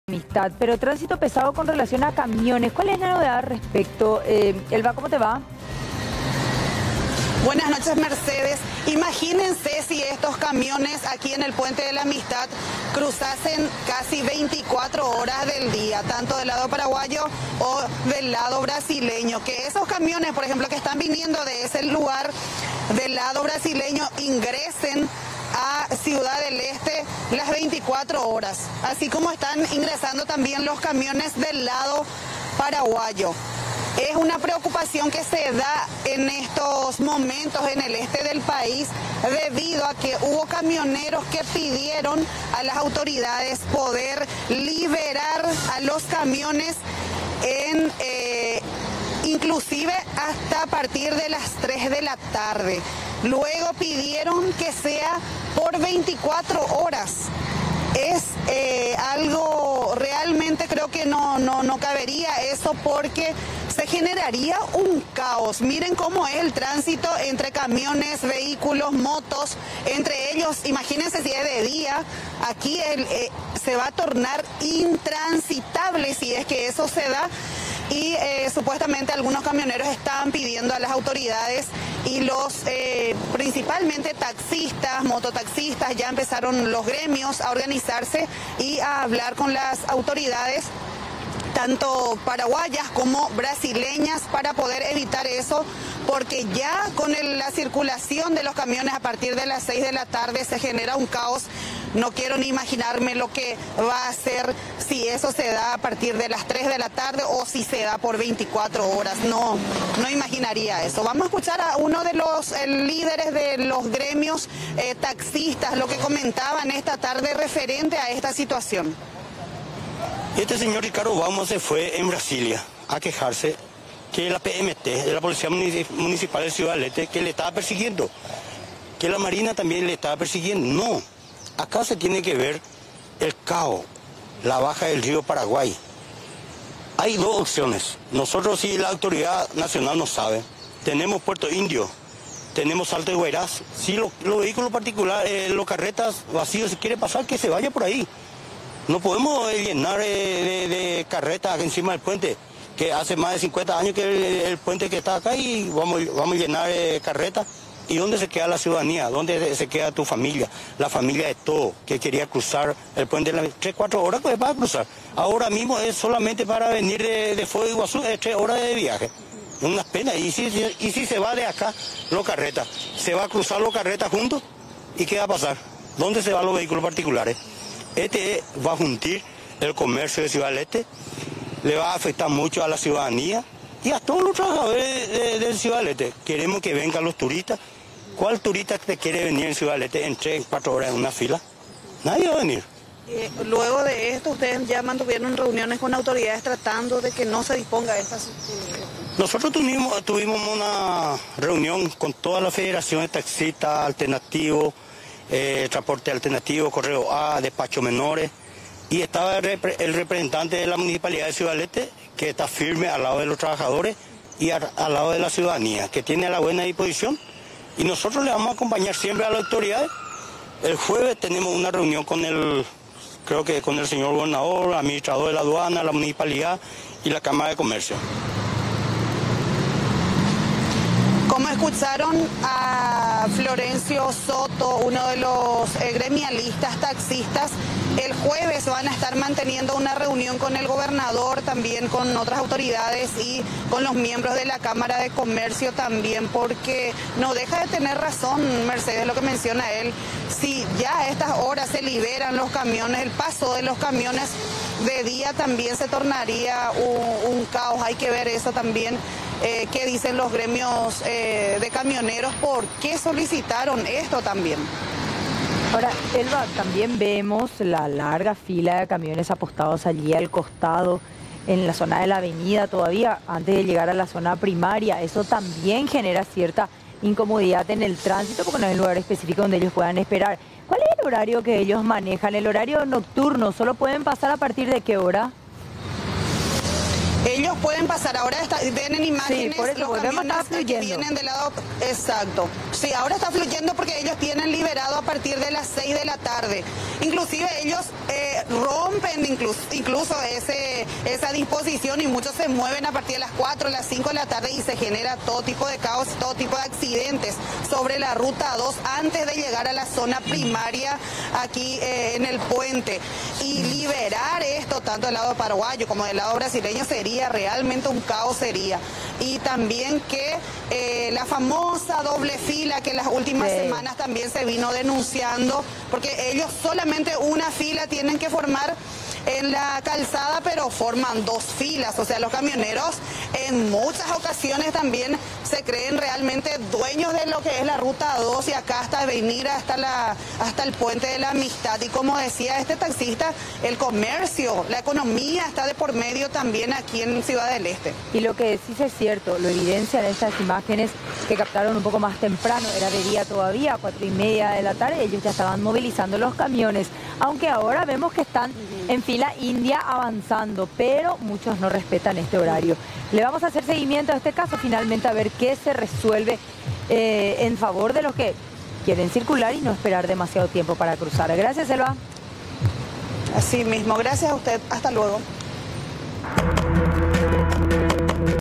Pretenden liberar el horario para los camiones
Un intenso tráfico se produce en horas de la noche en el Puente de la Amistad.

Según la información proporcionada, camioneros pidieron a las autoridades liberar a los camiones a partir de las 24 horas. Este hecho genera indignación a líderes de otros gremios, quienes afirman que esta situación podría generar un caos en el paso fronterizo y perjudicar al comercio.

"No podemos llenar el puente con camiones", afirmó Florencio Soto, gremialista taxista.

"Esto va a fundir el comercio de CDE, le va a afectar mucho a la ciudadanía y a todos los trabajadores, nadie va a venir", expresó.